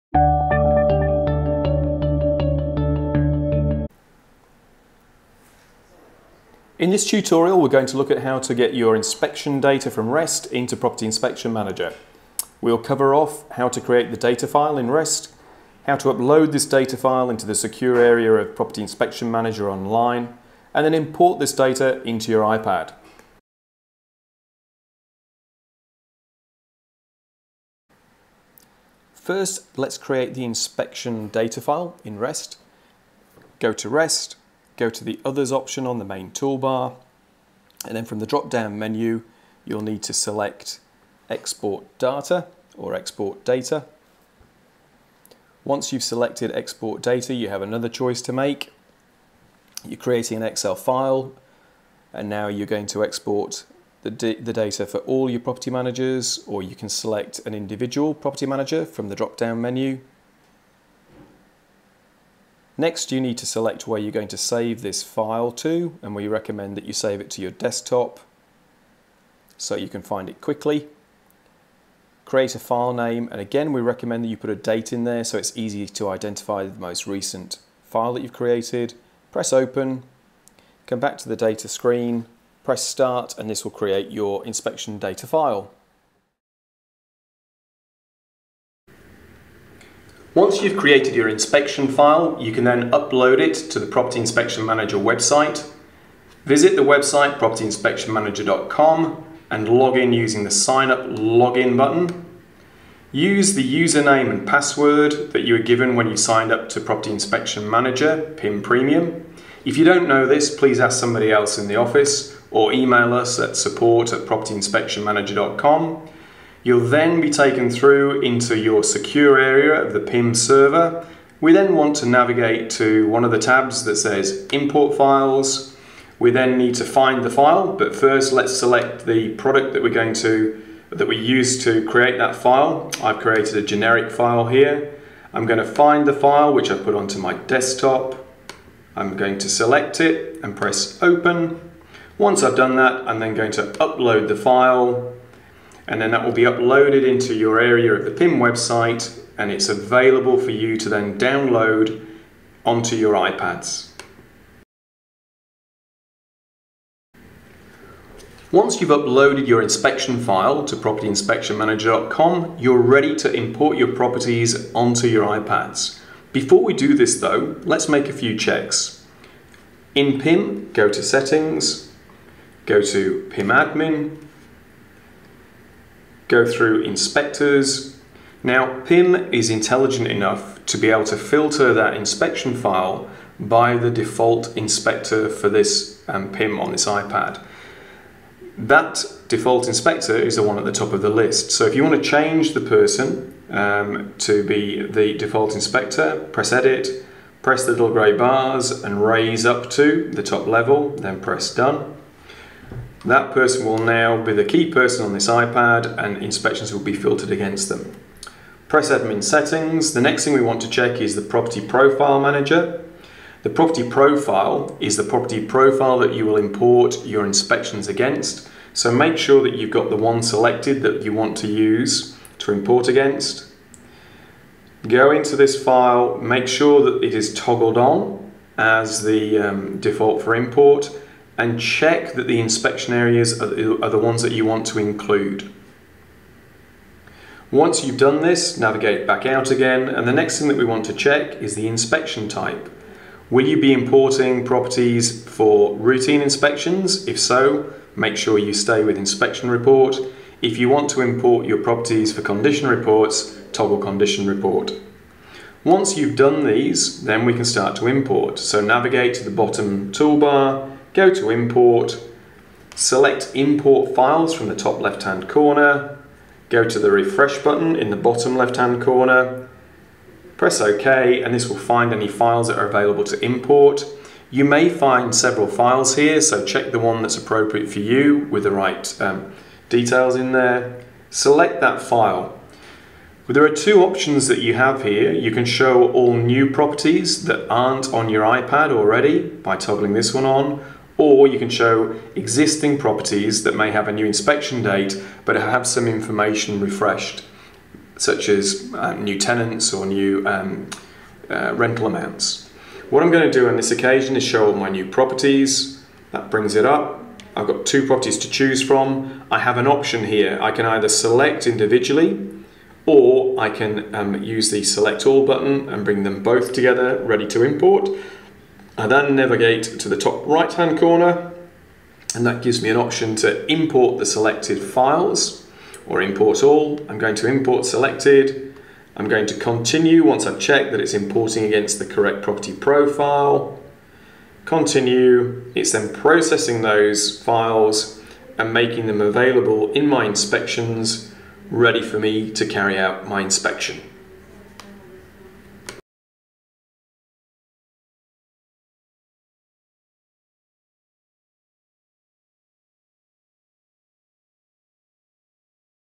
Partner Integration Series with REST
Find out how you can import data from your REST v14 property management software into PIM. This video includes
1. Exporting inspection data from REST.
2. Uploading Inspection data into your online account.
3. Import and Setting up your Inspection Data in PIM which includes updates to existing properties such as tenant changes, revised rental amounts and other details, where applicable.